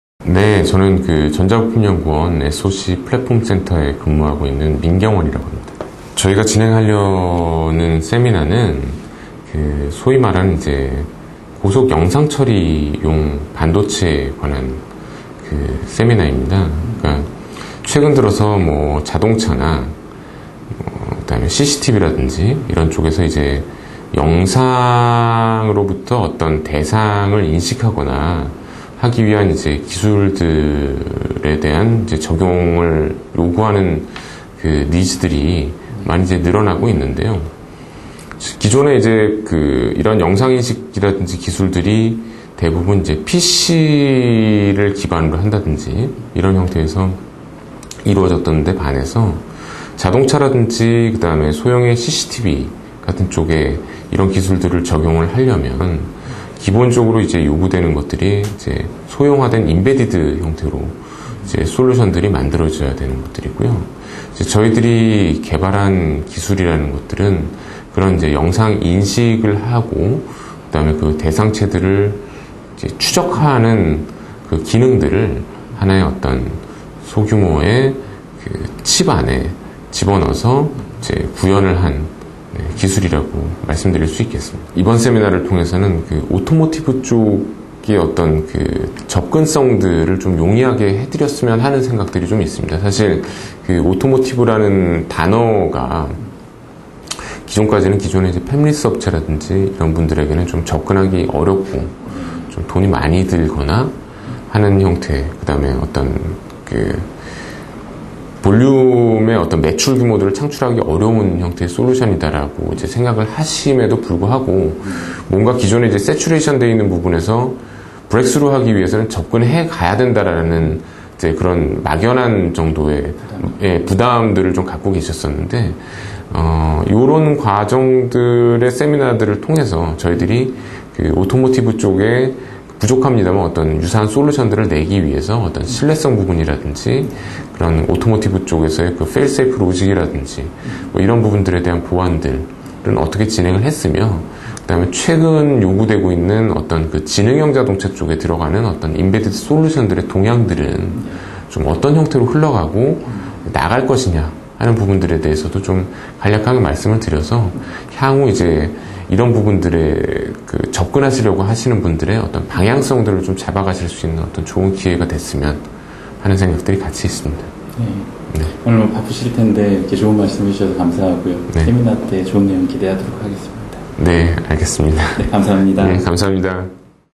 다중 카메라 기반 고속영상 SoC 플랫폼 기술
최근 급격한 차량의 전장화에 따른 센서의 채용 비율이 급속도로 증가되고 있습니다. 특히 전 세계적인 차량 안전기능 강화 요구에 따라 차량의 사고예방을 위한 충돌방지 지능형 센서 및 시스템에 대한 개발이 급속도로 진행되고 있는 상황입니다. 블랙박스 뿐 아니라 어라운드뷰 등의 용도로 차량 내 탑재되는 카메라 개수의 증가에 따라 카메라 기반의 영상인식 기술에 대한 중요도는 더욱 크게 증가하고 있는 추세입니다.

본 기술세미나를 통해서는 전자부품연구원과 한국 전자통신연구원이 공동 개발하고 있고 다채널로 동시 입력되는 영상으로부터 검지하고자 하는 대상 오브젝트(예:차량, 보행자 등)를 실시간 검지, 추적, 경보 하는 SoC기반 임베디드형 고속 비전인식 프로세싱 플랫폼인 다중카메라기반 고속 영상인식 SoC 플랫폼을 설명하고 차량용 반도체 및 영상기반의 다양한 어플리케이션을 구동하기 위한 시스템의 개발 동향을 파악해 봅니다.

나아가 위 기술이 알맞는 분야에 적용되어 더 큰 비즈니스로 확장하는 기업을 발굴 해내는 것 또한 한가지 목적입니다. 그럼 세미나에서 유익한 정보 많이 얻어가시기 바랍니다.